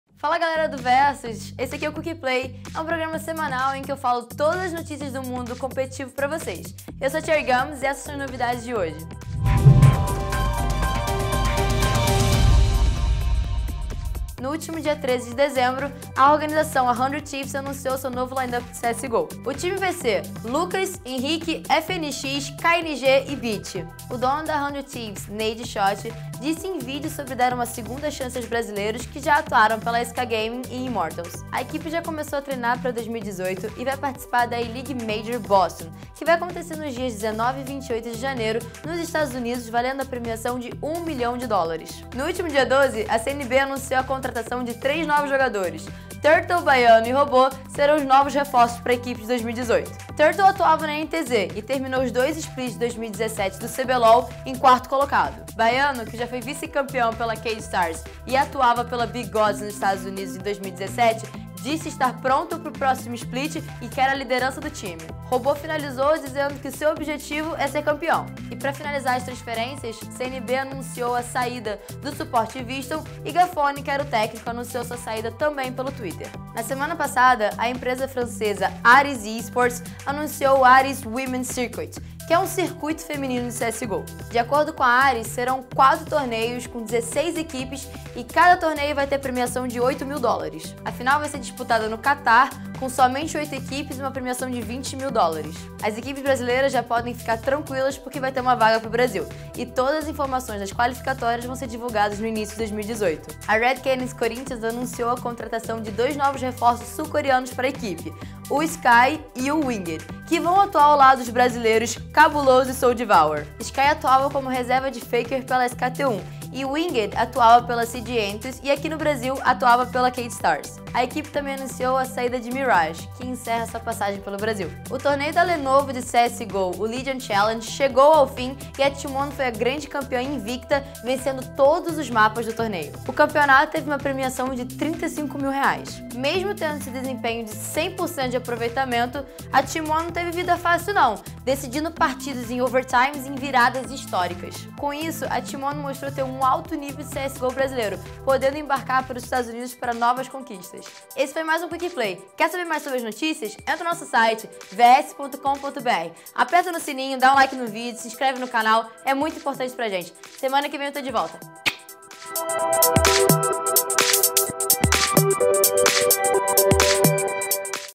COREANOS NA RED CANIDS CORINTHIANS E NOVO LINE-UP DA CNB - Quickplay | Versus
No Quickplay de hoje: Robo, Turtle e Baiano entram na CNB; Team One é campeã da Legion Challenge; Red Canids Corinthians anuncia os coreanos Winged e Sky.